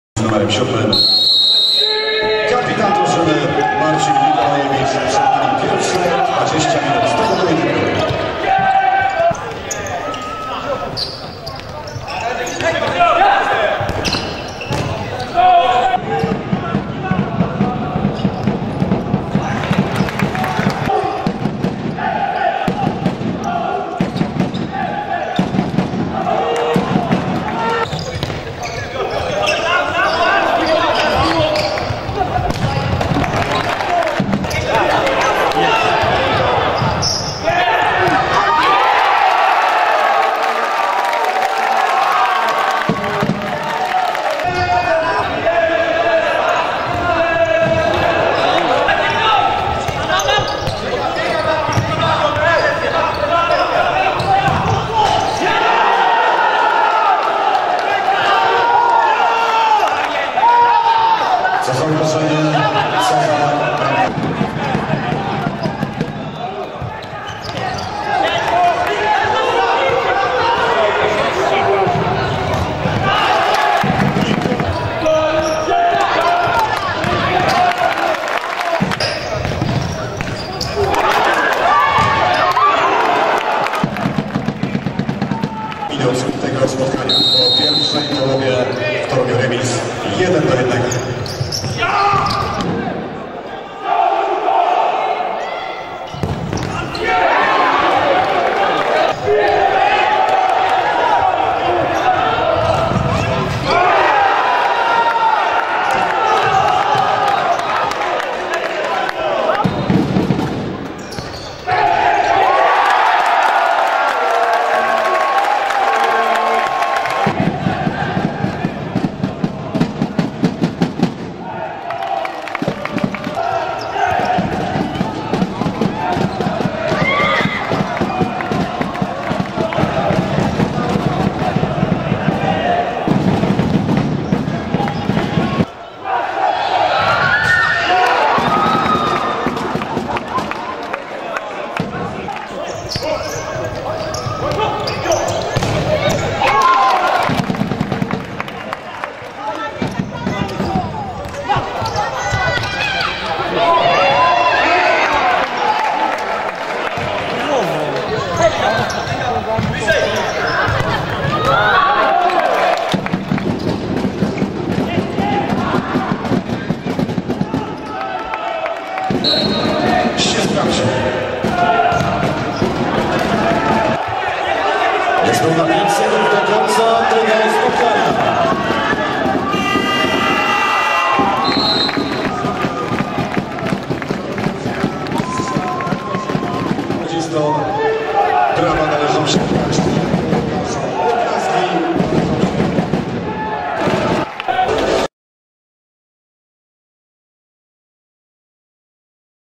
Poland League - Play-offs / Round 2 - FC Torun 2x2 Rekord Bielsko-Biala
في ثاني جولات الدور النهائي من الدوري البولندي للفوتسال, تورن يتعادل مع ريكورد و بنتيجة 2-2...